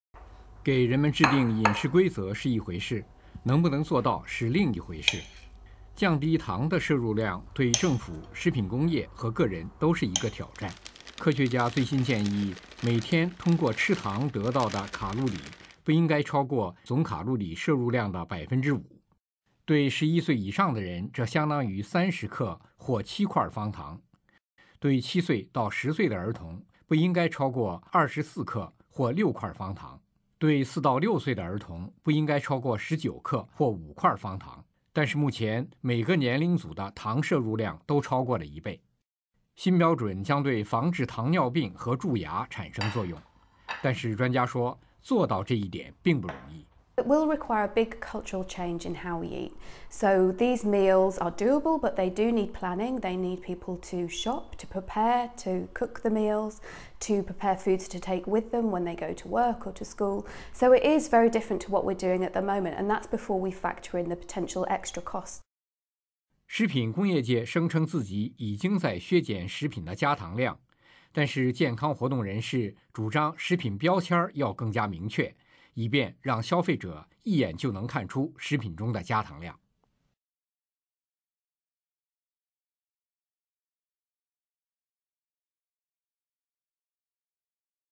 科学家建议限制饮食加糖分量
英国科学家提出公众饮食最新建议，在饮食中加入糖的成分不能超过人体总卡路里摄入量的5%。